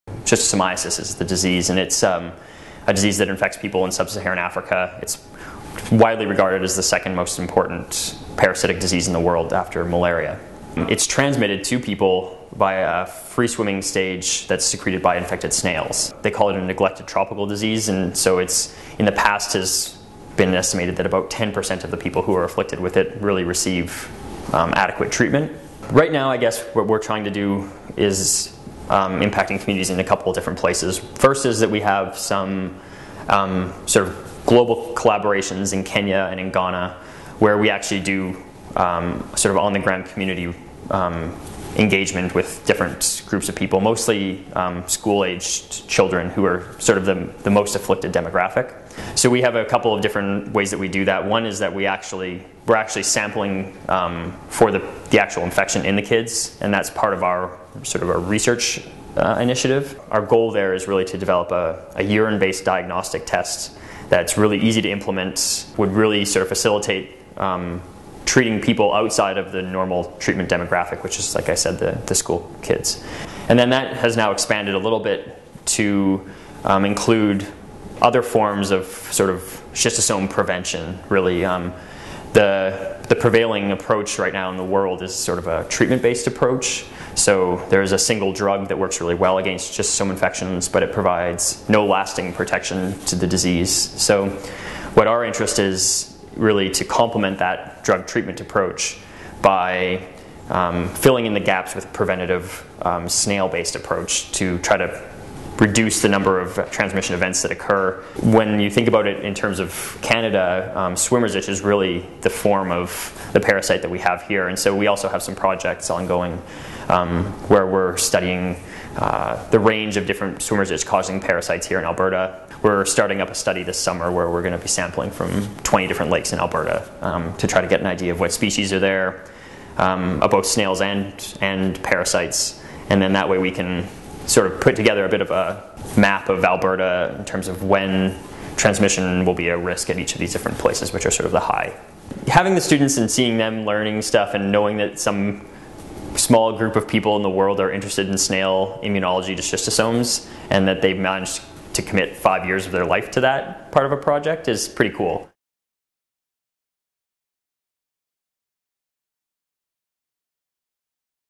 Snail research preventing illness and infection locally and globally
Schistosomiasis--it is a big word to represent a disease that is transmitted by one of the smallest  creatures you could think of, a snail. Patrick Hanington, assistant professor in the U of A School of Public Health, runs a research lab with a strong focus on schistosomiasis. "I am interested in learning how my research on this parasite applies to public health from a transmission and prevention standpoint," says Hanington.

"We are working on innovative research, publishing results and providing great learning opportunities to students."